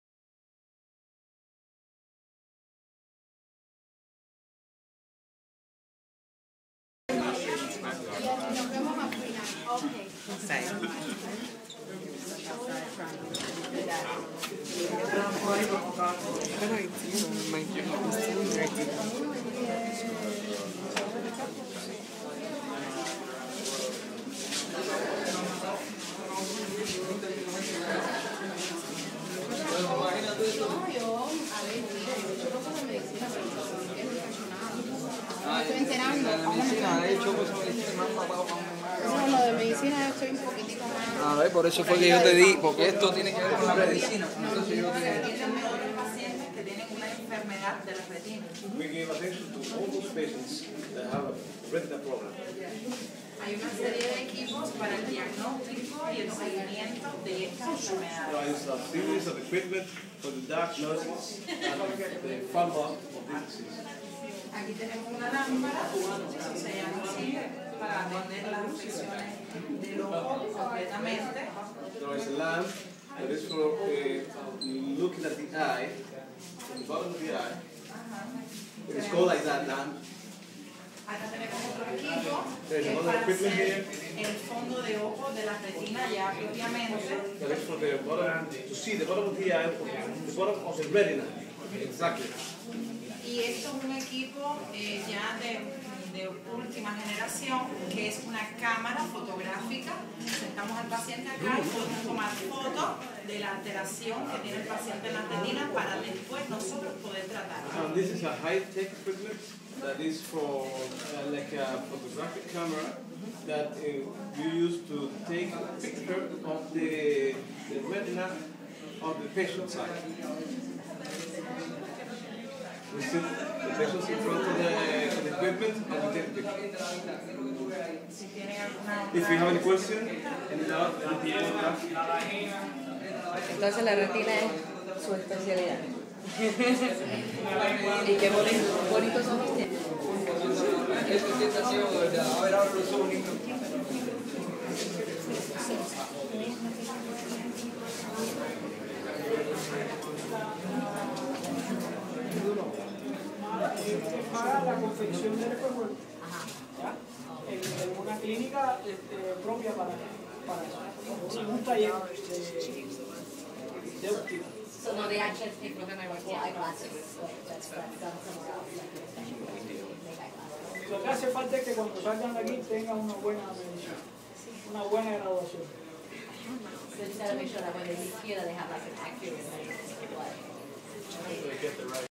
Venceremos Brigade tours a hospital
The VB tours an Eye Hospital in Cuba.  The rooms were open up and interactions between the doctors and brigadistas ensued.